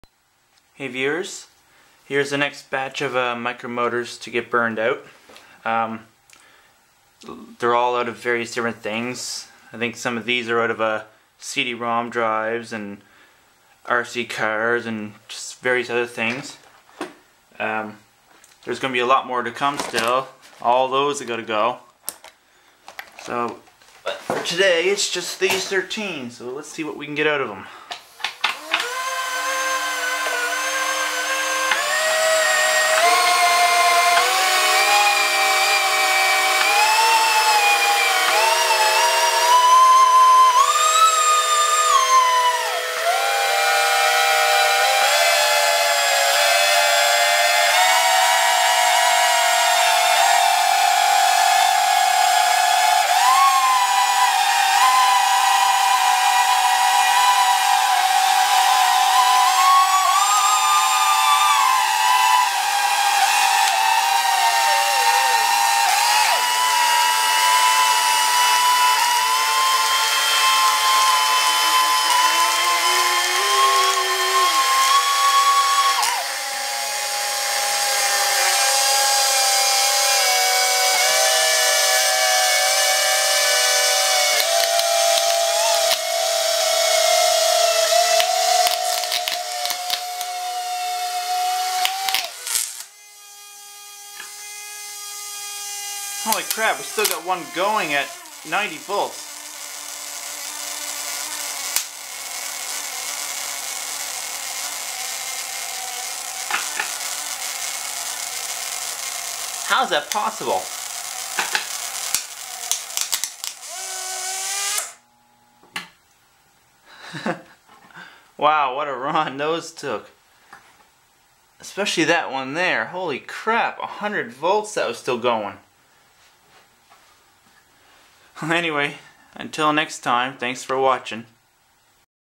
Micro Motors Overload 3
3rd video on burning out various micro motors with some impressive results.